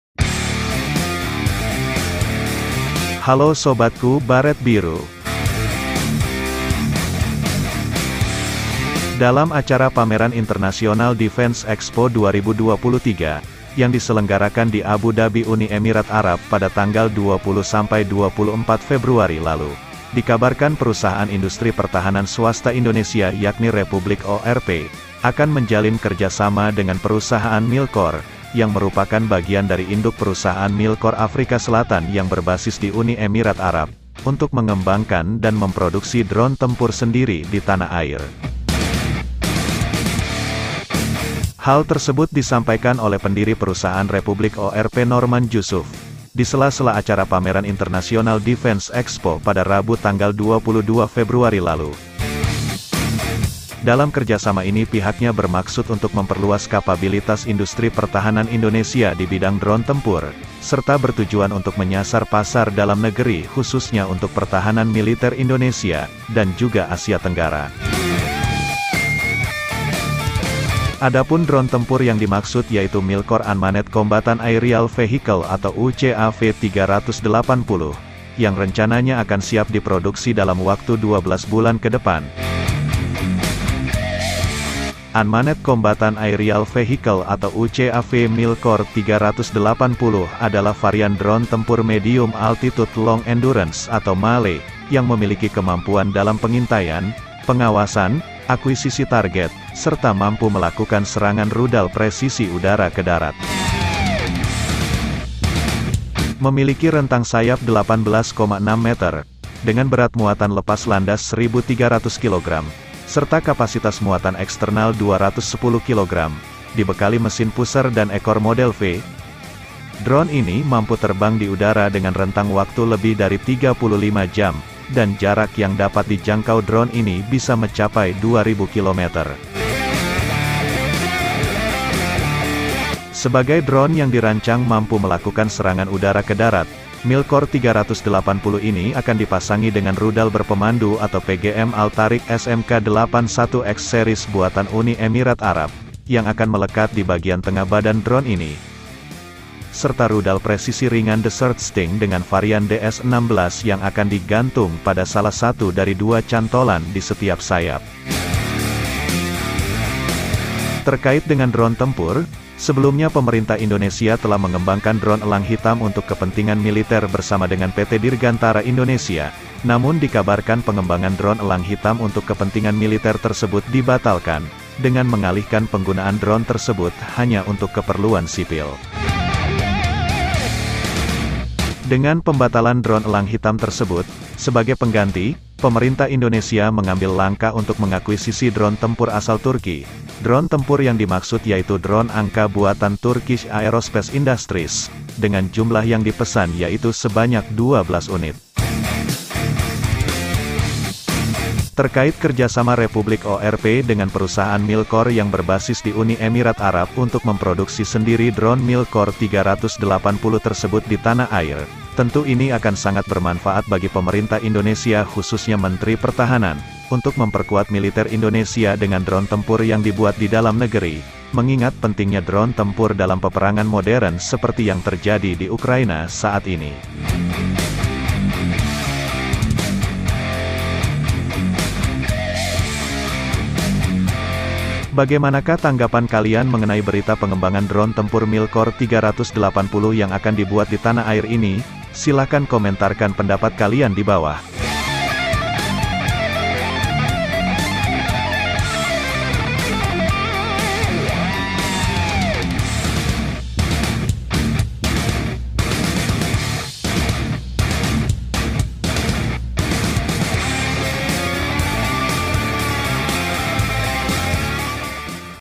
GAGAL KEMBANGKAN DRONE ELANG HITAM INDONESIA AKAN KEMBANGKAN DRONE MILKOR 380 DAN BORONG DRON ANKA
Dalam acara pameran Internasional Defence Expo 2023 yang diselenggarakan di Abu Dhabi Uni Emirat Arab pada 20 sampai dengan 24 Februari 2023, dikabarkan Perusahaan industri pertahanan swasta Indonesia yakni Republikorp, akan menjalin kerja sama dengan perusahaan Milkor yang merupakan bagian dari induk perusahaan Milkor Afrika selatan yang berbasis di Uni Emirat Arab, untuk mengembangkan dan memproduksi drone tempur sendiri di tanah air.

Adapun dorene tempur yang dimaksut yaitu Milkor Unmanned Combatant Aerial Vehicle atau  UCAV 380, yang rencanya akan siap diproduksi dalam waktu 12 bulan ke depan,
Unmanned Combatant Aerial Vehicle atau UCAV Milkor 380 adalah varian drone tempur Medium Altitude Long Endurance (MALE) yang memiliki kemampuan dalam pengintaian, pengawasan, akuisisi target serta mampu melakukan serangan rudal presisi udara ke darat.